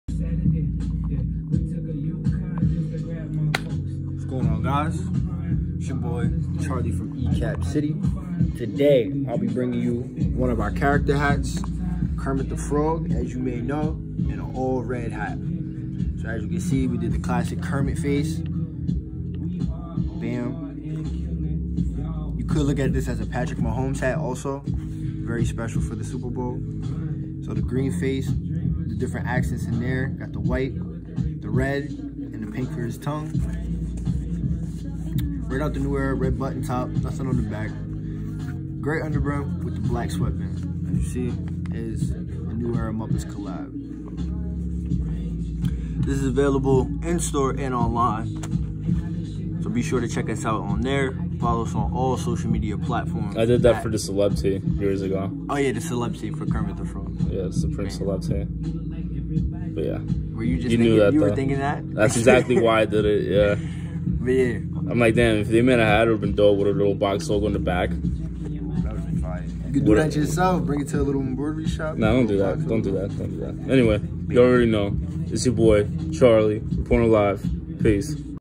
Kermit the Frog 59Fifty New Era Fitted Hats (The Muppets Red Gray Under Brim)
This Kermit 59Fifty New Era Fitted Hat was inspired by one of our favorite Supreme Celebrity Tee!  We did this to pay tribute not only to Kermit but to the internationally known brand Supreme! This Kermit the Frog New Era Fitted Cap is only available in red with a Gray under brim!